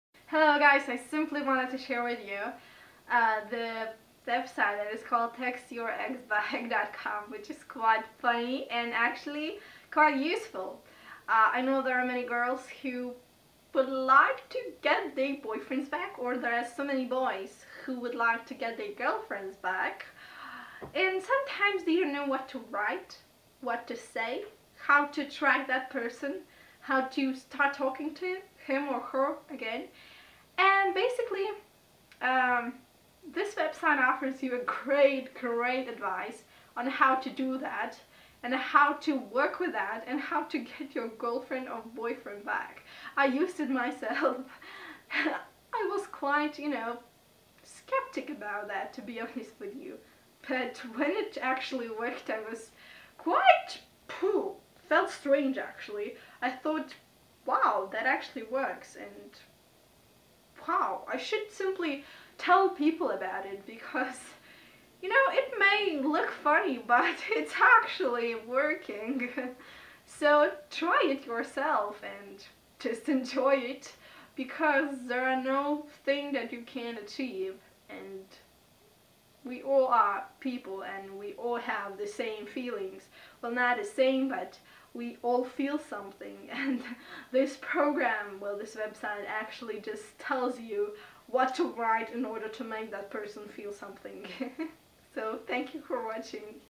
Text Your Ex Back Review ★ Text Your Ex Back By Michael Fiore
Text Your Ex Back creator by Michael Fiore, guidebook that helps people heal their broken heart after a failed relationship and get their ex-lover back without falling too much tear. If you are among people who regret after breaking up with your lover, you should try out Michael Fiore’s tips and techniques to attract and seduce the one you love once again. This program resulted from Michael Fiore’s years of researching and studying about psychology and human love. It is not the matter if you are a woman or a man because Text Your Ex Back PDF is divided into 2 separate programs: one for man and another for woman.